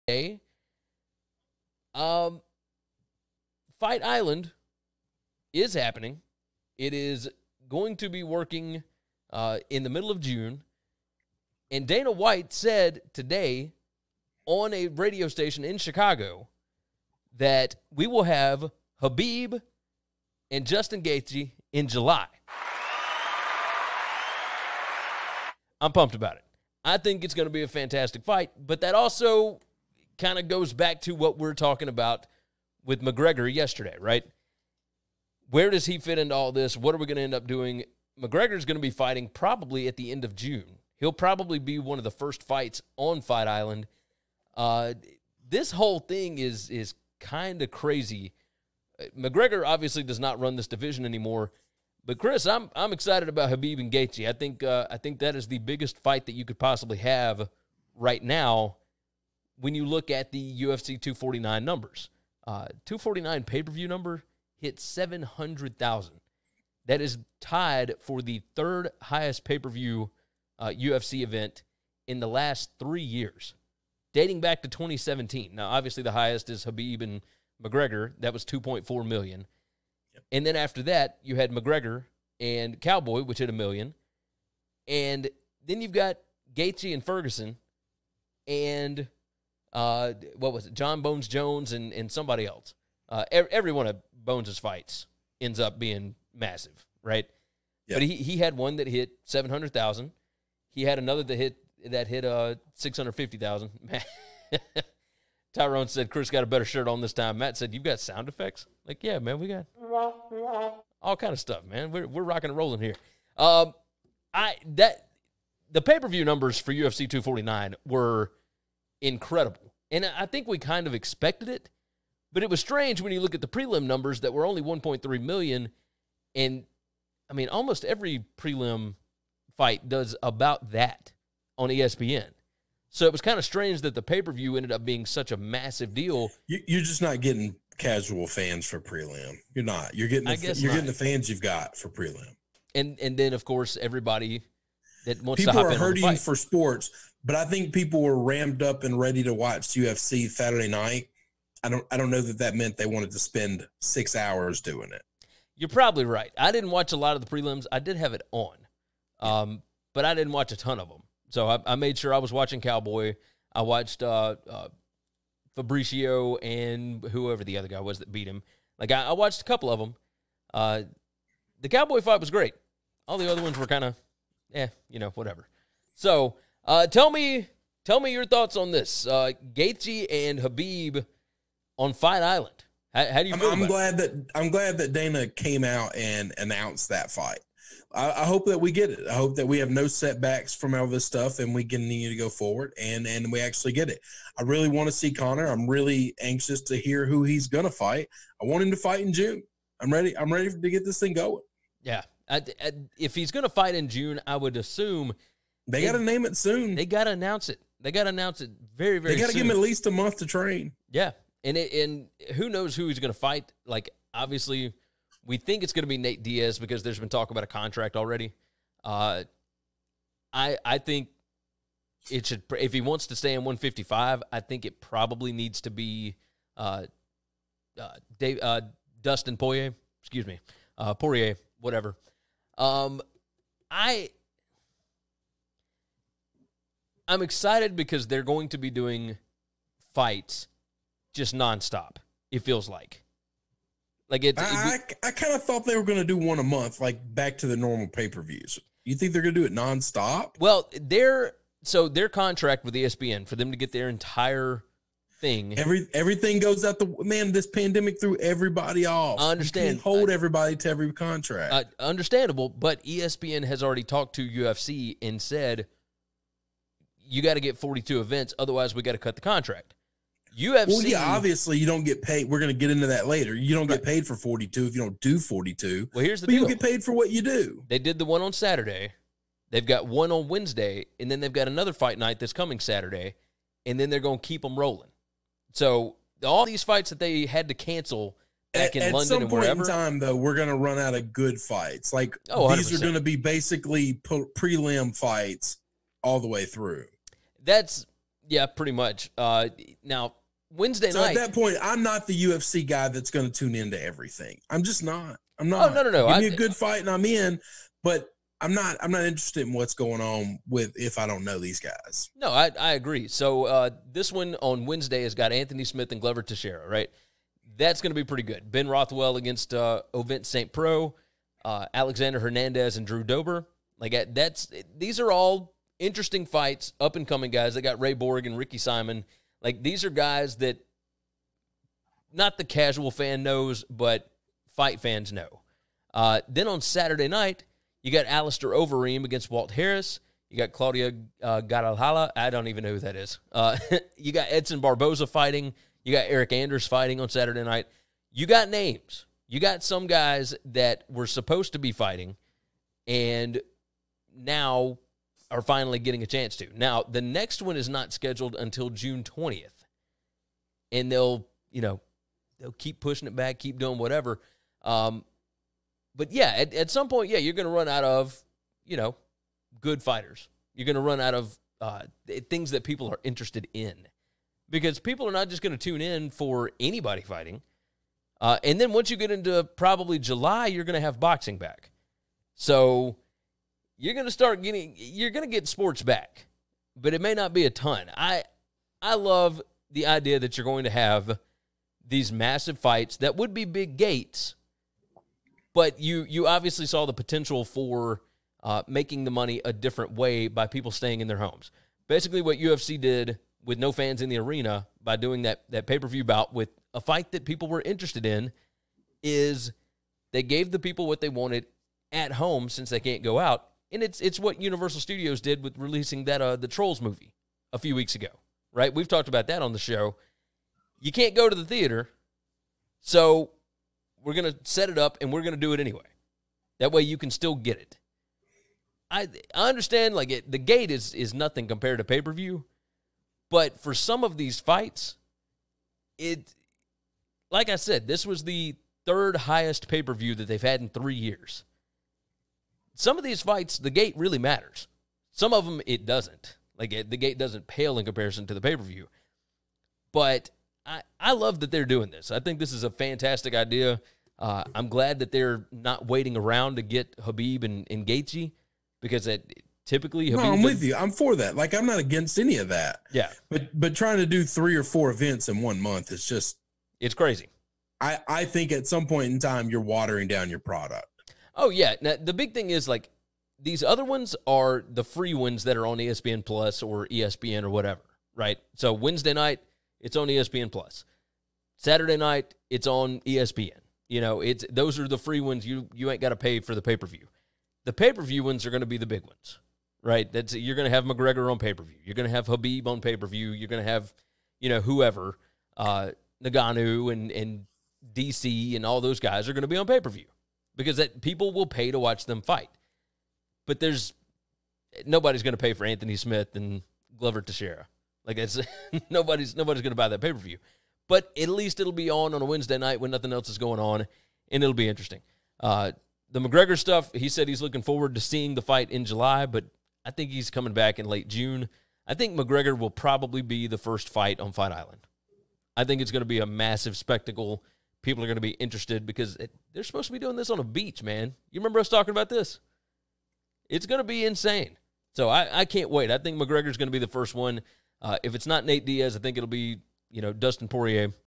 Khabib vs Gaethje in July on UFC's Fight Island? McGregor in June?
Khabib Nurmagomedov vs Justin Gaethje for the UFC Lightweight title happening on Fight Island in July?  Dana White let it slip on a Chicago radio station yesterday... which means we'll likely get a Conor McGregor fight in June.